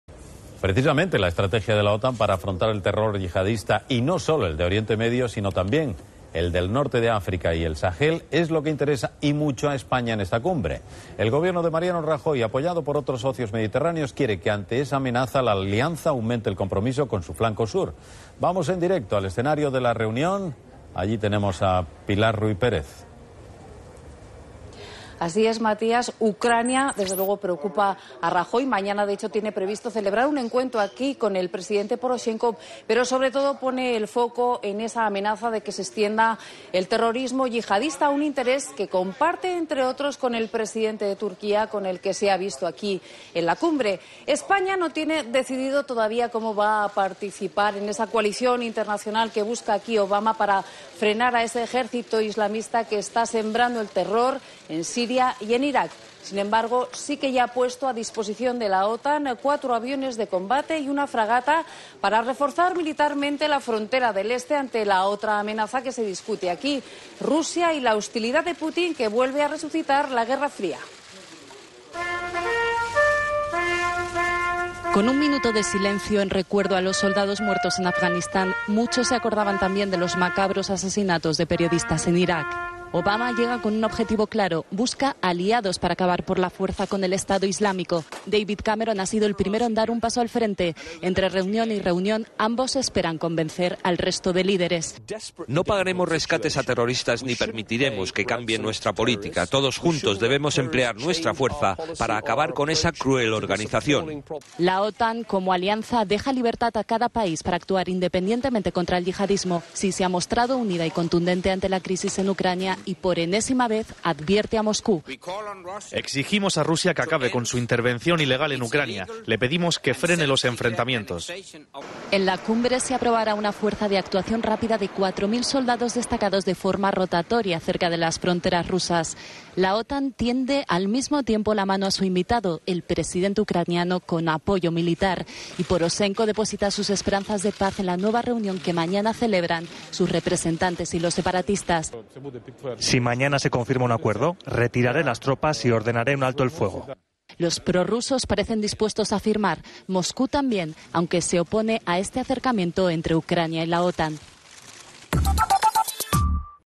La OTAN prepara su fuerza de acción inmediata en el Este para fin de año
Los 28 países acuerdan gastar más en defensa. Londres promete una aportación de 3.500 militares a la unidad